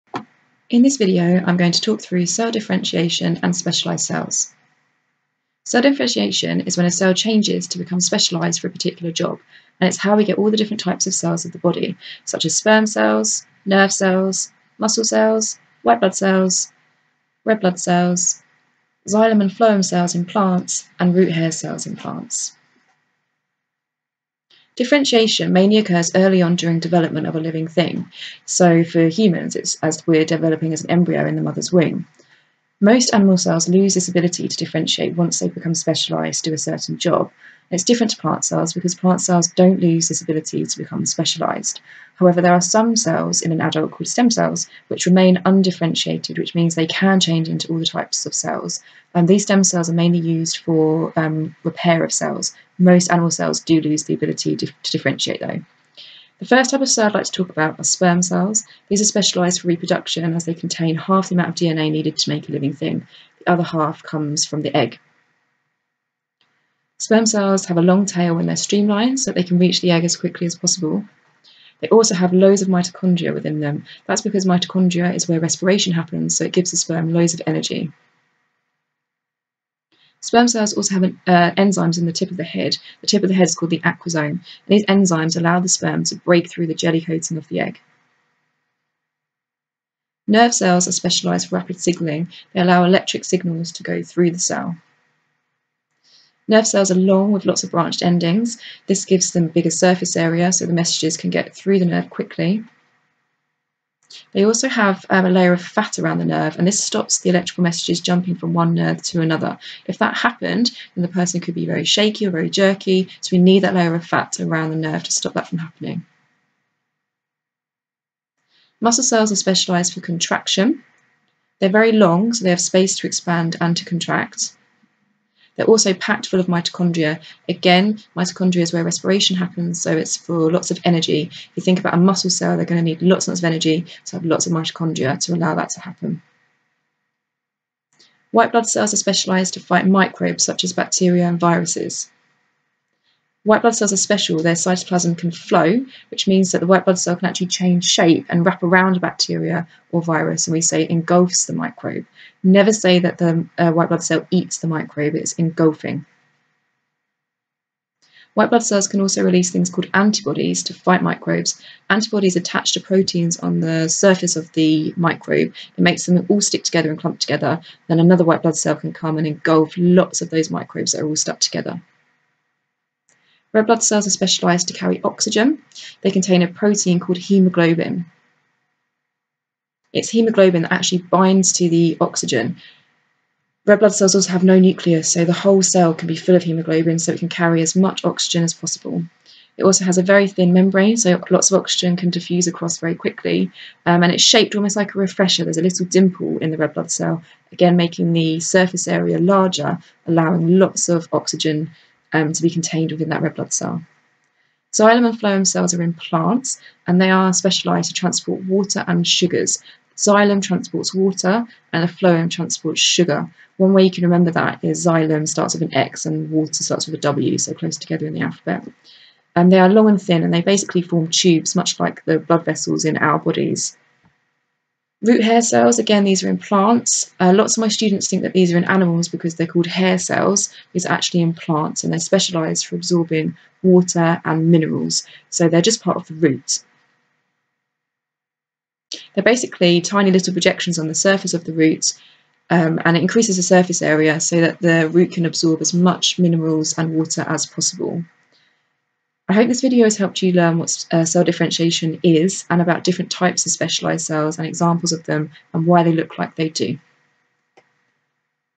Cell Differentiation and Specialised Cells - GCSE Biology
This video goes through what cell differentiation is, the main specialised cells you need to understand for GCSE Biology and why the cells look the way they do.